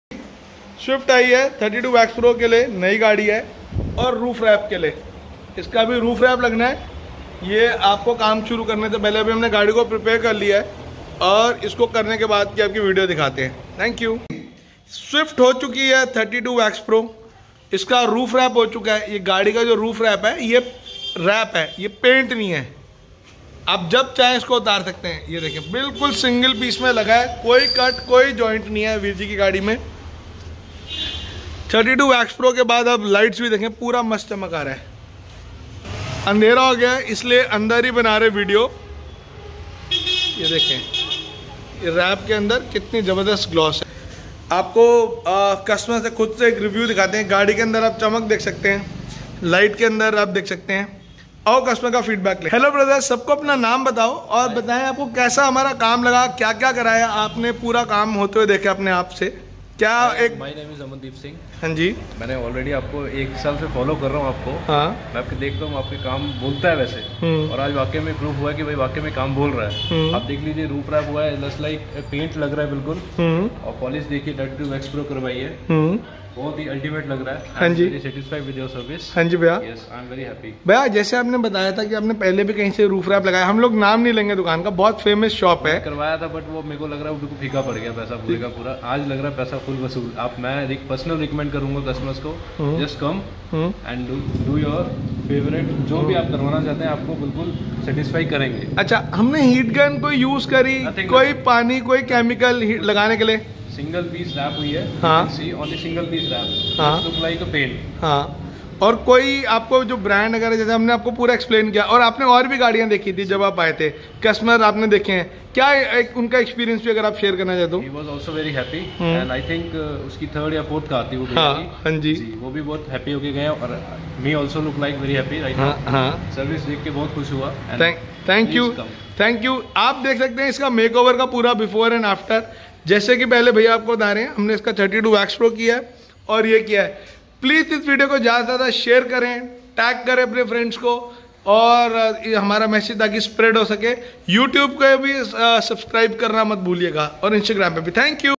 New White Swift 32waxpro And Ultra High Gloss Roof Wrap Without Use Of Heat Gun Connaught Care
Swift Got 32waxpro
32waxproplus
Nano Coating
Swift Got Ultra High Gloss Roof Wrap
3m wrap
Hexis
Avery Dennison
Connaught Care New Delhi
We Have No Branch